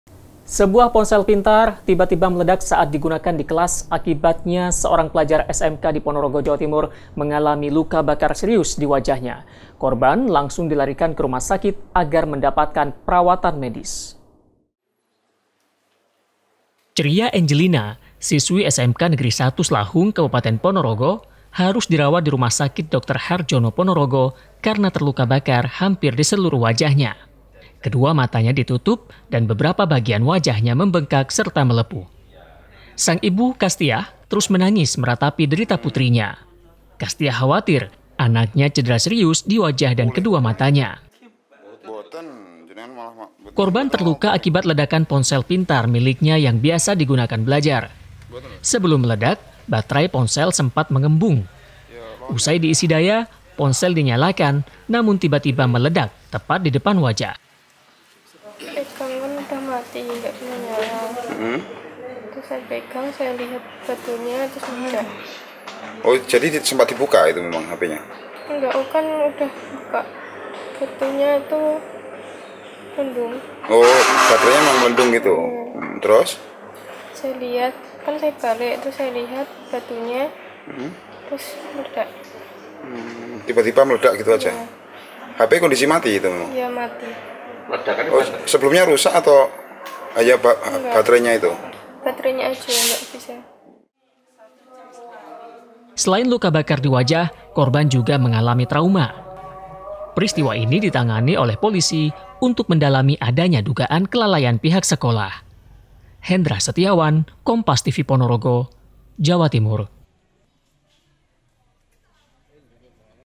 Ponsel Meledak, Wajah Siswi SMK Terluka Bakar
PONOROGO, KOMPAS.TV Sebuah ponsel pintar tiba-tiba meledak saat digunakan di kelas, akibatnya seorang pelajar SMK di Ponorogo Jawa Timur mengalami luka bakar serius di wajah. Korban langsung dilarikan ke rumah sakit agar mendapatkan perawatan medis.

Ceria Angelina, siswi SMK Negeri 1 Slahung Kabupaten Ponorogo harus dirawat di rumah sakit dr Harjono Ponorogo, karena terluka bakar hampir di seluruh wajahnya. Kedua matanya ditutup dan beberapa bagian wajahnya membengkak serta melepuh.

Sang ibu, Kastiah terus menangis meratapi derita putrinya. Kastiah khawatir anaknya cedera serius di wajah dan kedua matanya.

Baca Juga Rampas HP Siswi SMP, 2 Jambret Babak Belur Dihajar Warga di

Korban terluka akibat ledakan ponsel pintar miliknya yang biasa digunakan belajar. Sebelum meledak, baterai ponsel sempat melembung. Usai diisi daya, ponsel dinyalakan, namun tiba-tiba meledak tepat di depan wajah.

Selain luka bakar di wajah, korban juga mengalami trauma. Peristiwa itu ditangani oleh polisi untuk mendalami adanya dugaan kelalaian pihak sekolah.